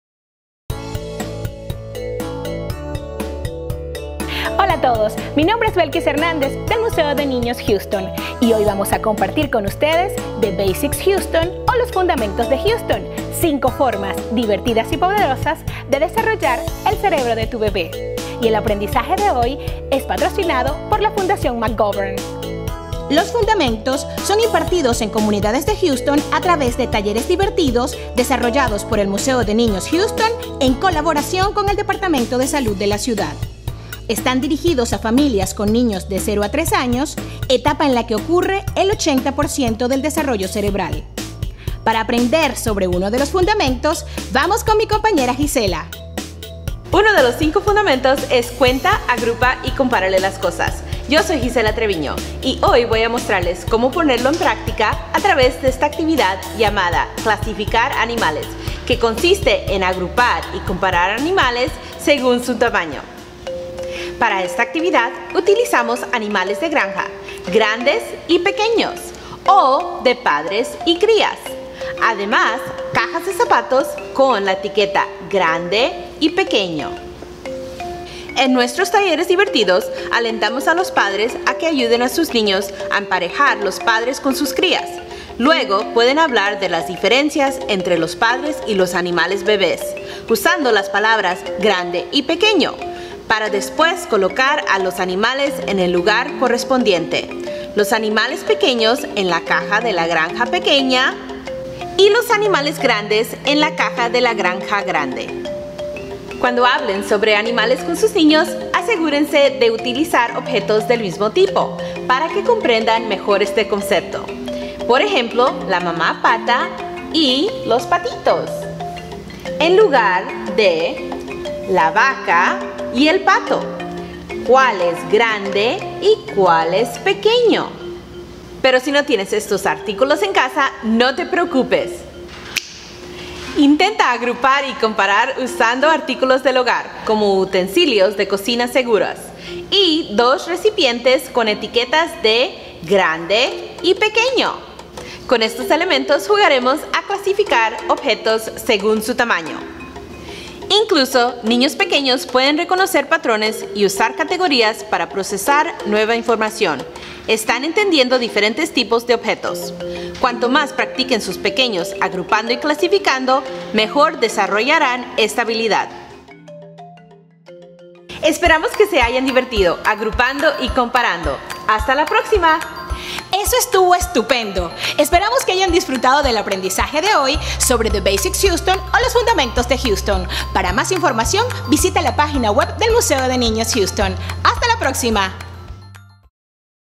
Cuenta, Agrupa y Compárale las Cosas: Clasificar Animales- The Basics Houston-Early Childhood Corner
"Los Fundamentos de Houston" son cinco maneras potentes y divertidas de desarrollar el cerebro de tu bebé.  En este episodio, "Cuenta, agrupa y compárale las cosas, " nuestras educadoras explican cómo clasificar objetos por tamaño, ayudan a los bebés a reconocer patrones y usar categorías para procesar nueva información.
The Basics Houston are five fun, powerful ways to build your baby's brain.  In this episode, "Count, Group and Compare," Children's Museum Houston educators explain how categorizing objects by size helps develop your young one's abilities to recognize patterns and process new information.